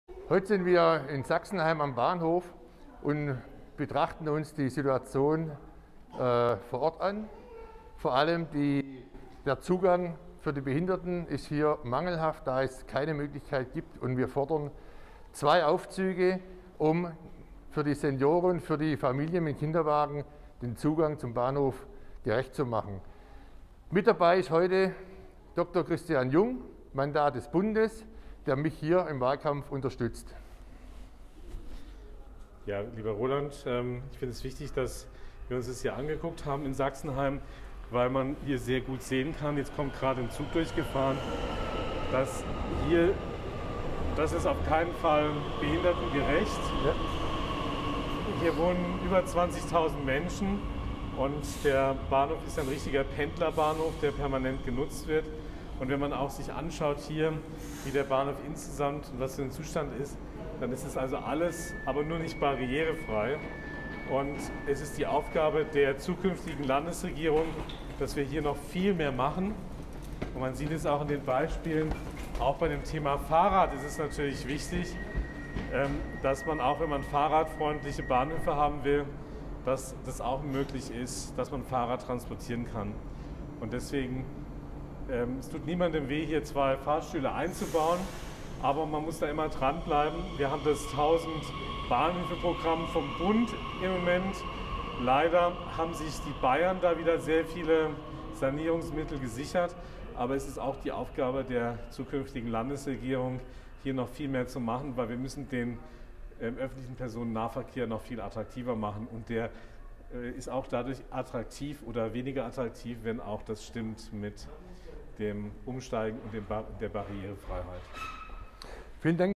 Barrierefreier Ausbaus am Bahnhof Sachsenheim dringend erforderlich
Heute unterhalten sich Roland Zitzmann und Christian Jung MdB, Verkehrsexperte der FDP-Bundestagsfraktion, über die dringende Notwendigkeit eines barrierefreien Ausbaus des Bahnhofs in Sachsenheim. Die beiden Politiker sind sich einig, dass der Einbau von 2 Aufzügen schnellstens erfolgen muss.